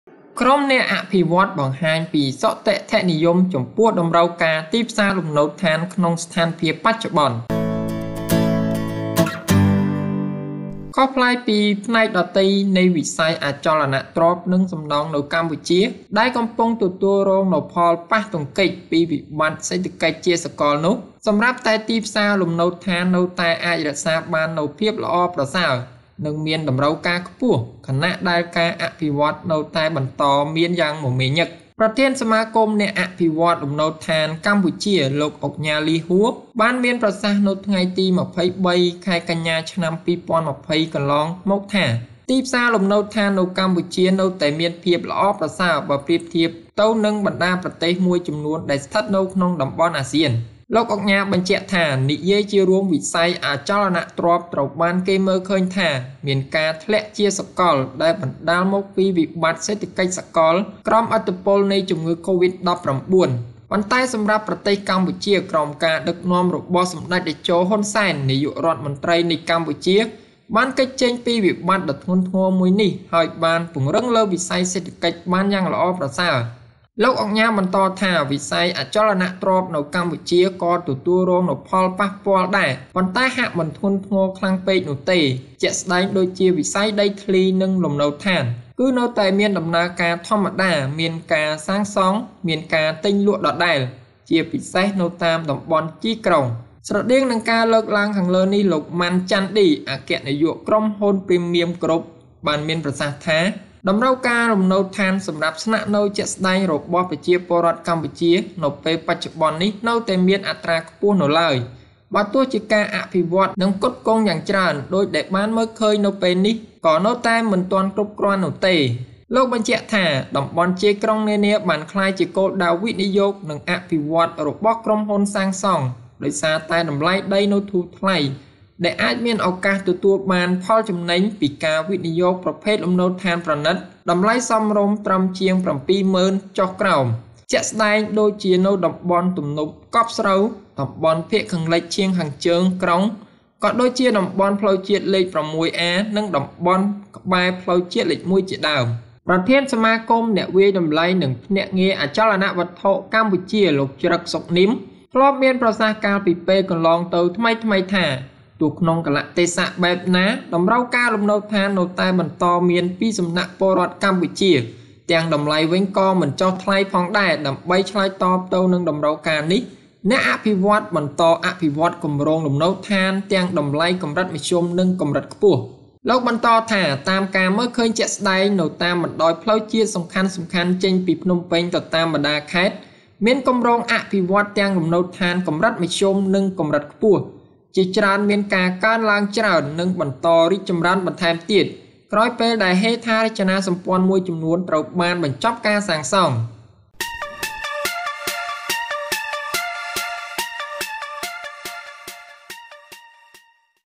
​អ្នក​អភិវឌ្ឍន៍​បង្ហាញ​ពី​សុទិដ្ឋិនិយម​ចំពោះ​តម្រូវការ​ទីផ្សារ​លំនៅដ្ឋានក្នុងស្ថានភាព​បច្ចុប្បន្ន
ខុសប្លែកពីផ្នែកដទៃនៃវិស័យអចលនទ្រព្យ និងសំណង់នៅកម្ពុជា ដែលកំពុងទទួលរងនូវផលប៉ះទង្គិចពីវិបត្តិសេដ្ឋកិច្ចជាសកលនោះ សម្រាប់តែទីផ្សារលំនៅដ្ឋាននៅតែអាចរក្សាបាននូវភាពល្អប្រសើរ និងមានតម្រូវការខ្ពស់ ខណៈដែលការអភិវឌ្ឍន៍នៅតែបន្តមានយ៉ាងមមាញឹក។

ប្រធានសមាគមអ្នកអភិវឌ្ឍន៍លំនៅដ្ឋានកម្ពុជា លោកឧកញ៉ា លី ហួរ បានមានប្រសាសន៍នៅថ្ងៃទី២៣ ខែកញ្ញា ឆ្នាំ២០២០ថា ទីផ្សារលំនៅដ្ឋាននៅកម្ពុជា នៅតែមានភាពល្អប្រសើរបើប្រៀបធៀបទៅនឹងបណ្តាប្រទេសមួយចំនួនដែលស្ថិតនៅក្នុងតំបន់អាស៊ាន។

លោកឧកញ៉ាបានមានប្រសាសន៍បញ្ជាក់ថា “និយាយជារួម វិស័យអចលនទ្រព្យត្រូវបានគេមើលឃើញថាមានការធ្លាក់ជាសាកលដែលបណ្តាលមកពីវិបត្តិសេដ្ឋកិច្ចសាកលក្រោមឥទ្ធិពលនៃជំងឺCovid-19 ប៉ុន្តែសម្រាប់ប្រទេសកម្ពុជា ក្រោមការដឹកនាំរបស់សម្ដេចតេជោ ហ៊ុន សែន នាយករដ្ឋមន្ត្រីនៃកម្ពុជា បានគេចចេញពីវិបត្តិដ៏ធ្ងន់ធ្ងរមួយនេះ ហើយបានពង្រឹងលើវិស័យសេដ្ឋកិច្ចបានយ៉ាងប្រសើរ”។

លោកបន្តថា “វិស័យអចលនទ្រព្យនៅកម្ពុជា ក៏ទទួលរងនូវផលប៉ះពាល់ដែរ ប៉ុន្តែហាក់មិនធ្ងន់ធ្ងរខ្លាំងពេកនោះទេ ជាក់ស្ដែងដូចជាវិស័យដីធ្លី និងលំនៅដ្ឋាន គឺនៅតែមានដំណើរការធម្មតា មានការសាងសង់ មានការទិញ-លក់ ដដែល ជាពិសេសនៅតាមតំបន់ជាយក្រុង”។

ស្រដៀងនឹងការលើកឡើងខាងលើនេះ លោក ម៉ាន់ ច័ន្ទឌី អគ្គនាយកក្រុមហ៊ុន PREMIUM Group បានមានប្រសាសន៍ថា “តម្រូវការលំនៅដ្ឋានសម្រាប់ស្នាក់នៅជាក់ស្ដែងរបស់ប្រជាពលរដ្ឋកម្ពុជានៅពេលបច្ចុប្បន្ននេះនៅតែមានក្នុងអត្រាខ្ពស់នៅឡើយ បើទោះជាការអភិវឌ្ឍន៍ និងផ្គត់ផ្គង់យ៉ាងច្រើនដូចដែលបានឃើញនៅពេលនេះក៏នៅតែមិនទាន់គ្រប់នោះទេ”។

លោកបញ្ជាក់ថា “តំបន់ជាយក្រុងនានា បានក្លាយជាគោលដៅវិនិយោគ និងអភិវឌ្ឍន៍របស់ក្រុមហ៊ុនសាងសង់ ដោយសារតែតម្លៃដីនៅធូរថ្លៃ ដែលអាចមានឱកាសទទួលបានចំណេញពីការវិនិយោគប្រភេទលំនៅដ្ឋានប្រណីតតម្លៃសមរម្យត្រឹមជាង៧ ម៉ឺនចុះក្រោម។ ជាក់ស្ដែងដូចជានៅតំបន់ទំនប់កប់ស្រូវ តំបន់ភាគខាងលិចឈៀងខាងជើងក្រុង ក៏ដូចជាតំបន់ផ្លូវជាតិលេខ៦A និងតំបន់ក្បែរផ្លូវជាតិលេខ១ ជាដើម”។

ប្រធានសមាគមអ្នកវាយតម្លៃ និងភ្នាក់ងារអចលនវត្ថុកម្ពុជា លោក ច្រឹក សុខនីម ធ្លាប់មានប្រសាសន៍កាលពីពេលកន្លងទៅថ្មីៗថា ទោះក្នុងកាលៈទេសៈបែបណា តម្រូវការលំនៅឋាន នៅតែបន្តមានពីសំណាក់ពលរដ្ឋកម្ពុជា ទាំងតម្លៃវិញក៏មិនចុះផងដែរ ដើម្បីឆ្លើយតបទៅតម្រូវការនេះ អ្នកអភិវឌ្ឍន៍ បន្តអភិវឌ្ឍន៍គម្រោងលំនៅដ្ឋានទាំងតម្លៃកម្រិតមធ្យម និងកម្រិតខ្ពស់។

លោកបន្តថា “តាមការមើលឃើញជាក់ស្តែង នៅតាមបណ្តោយផ្លូវជាតិសំខាន់ៗចេញពីភ្នំពេញទៅតាមបណ្តាខេត្ត មានគម្រោងអភិវឌ្ឍន៍ទាំងលំនៅឋានកម្រិតមធ្យម និងកម្រិតខ្ពស់ ជាច្រើនមានការកើនឡើងច្រើន និងបន្តរីកចំរើនបន្ថែមទៀតក្រោយពេលដែលហេដ្ឋារចនាសម្ព័ន្ធមួយចំនួនត្រូវបានបញ្ចប់ការសាងសង់”៕